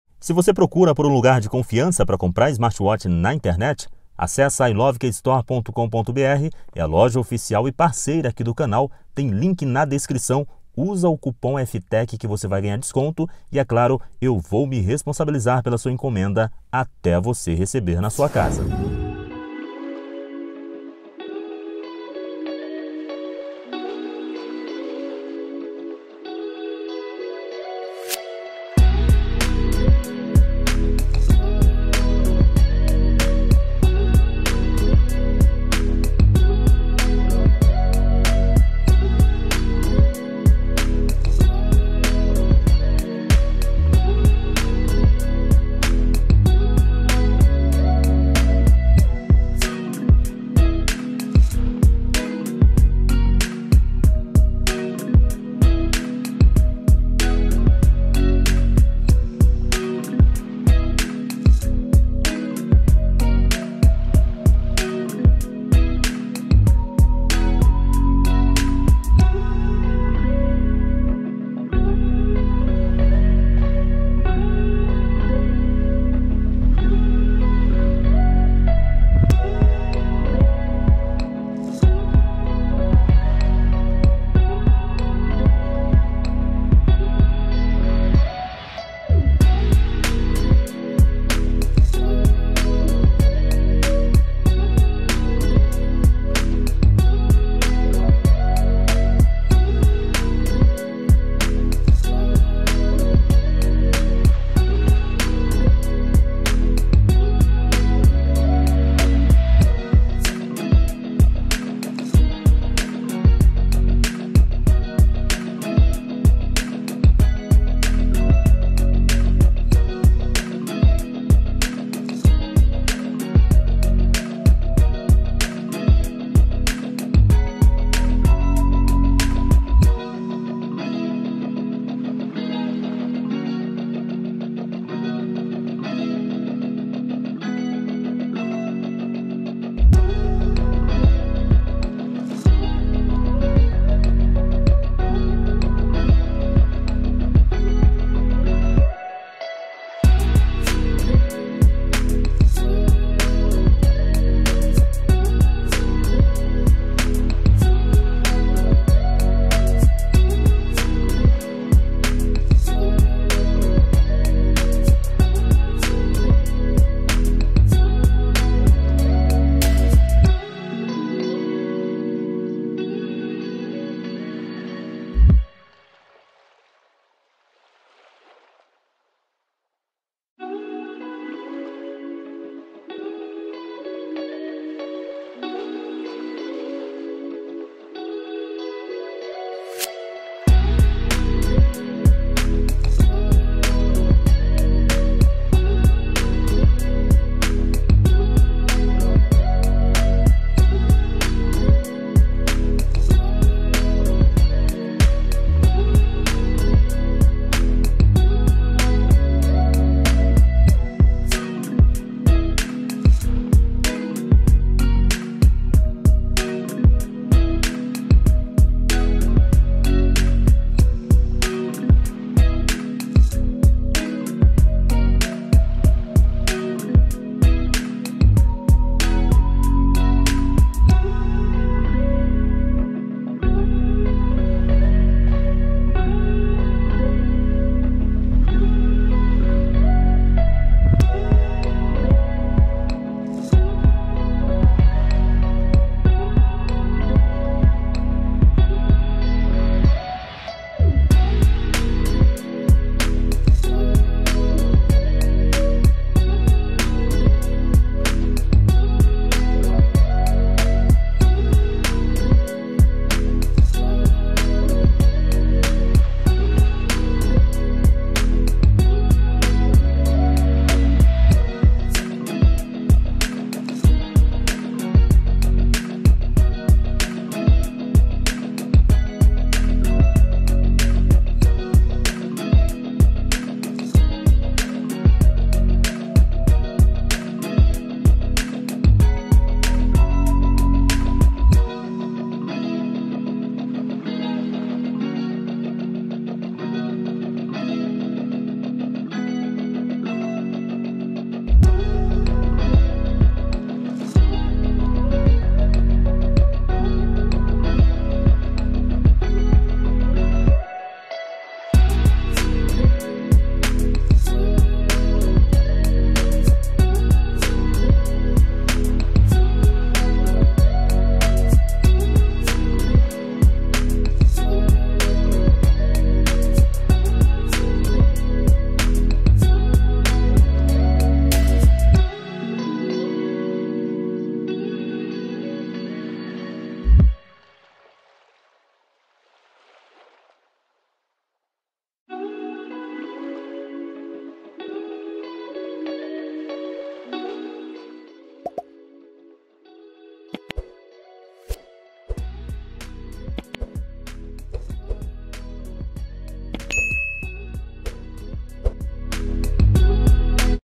COMPARATIVO: IWO S7 vs IWO S7 MAX! Qual vale mais a pena? Tudo em detalhes.
Comparativo IWO S7 Max Series 7 vs IWO S7, acompanhe em detalhes nesse vídeo.

Abraço!